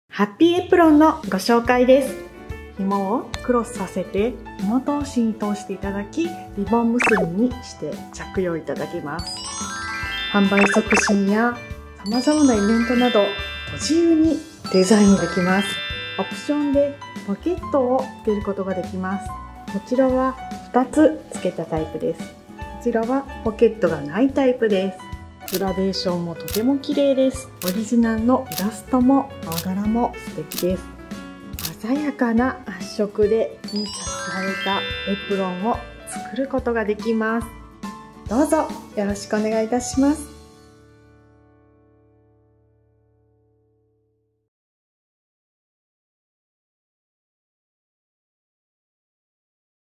ハッピィエプロン【かんばん工房】
1枚から作れるフルオーダー・フルカラーのオリジナルエプロン！写真やイラスト、言葉を入れてインパクトのあるエプロンが作れます。少人数での展示会出展やお店でのオリジナルユニフォームとして、母の日や誕生日など記念日のお祝い、開店祝いのプレゼントとしても人気です。

PR力のある「ハッピィエプロン」は、販売促進はもちろん、さまざまな場所で活躍いたします。
●農産物・水産物のPRイベント　●観光PRや産直市のイベント　●展示会スタッフユニフォーム
●オリジナル販売グッズ　●誕生日プレゼント　●母の日プレゼント
店舗や展示会の販促用のオリジナルデザイン、お子様の写真やペットの写真、お好きなイラストでの作成もできます。

布にプリントし、縫製するので、「エプロン全体」にデザインができ、オンリーワンのエプロンが作成可能。

写真やイラストを美しく再現いたします。１枚ずつ文字や写真の差し替え可能です。
完全データでの作成はもちろん、当社のデザイナーがオシャレな目を引くデザインに仕上げる事もできます。

詳細は、「かんばん工房　ハッピィエプロン」をご覧ください。
愛媛県宇和島市吉田町で、自社生産しています。

【かんばん工房】

〒799-3701　愛媛県宇和島市吉田町北小路乙19-2　佐川印刷株式会社 吉田工場
Sakawa Digital Printing Factory